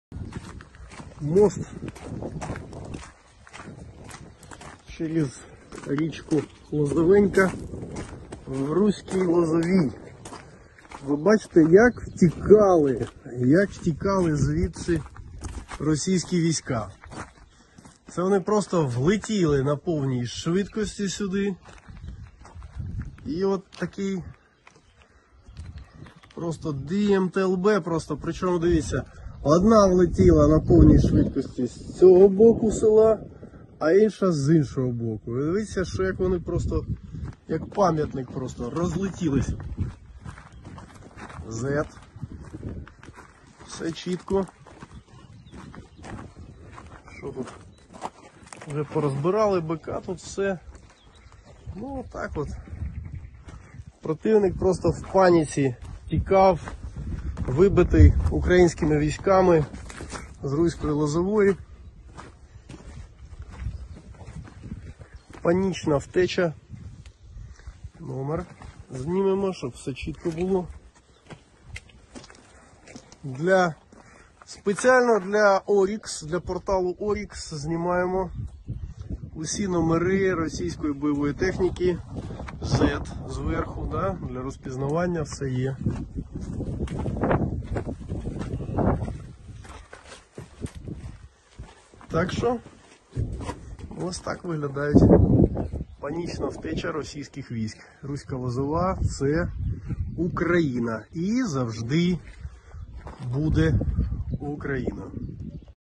Русские драпают из Харьковской области и не замечают мостов, думают что умеют летать
Россйские военные решили, что умеют летать на бронетехнике!

В Русской Лозовой Харьковской области орки спешили так, что не заметили взорванного моста.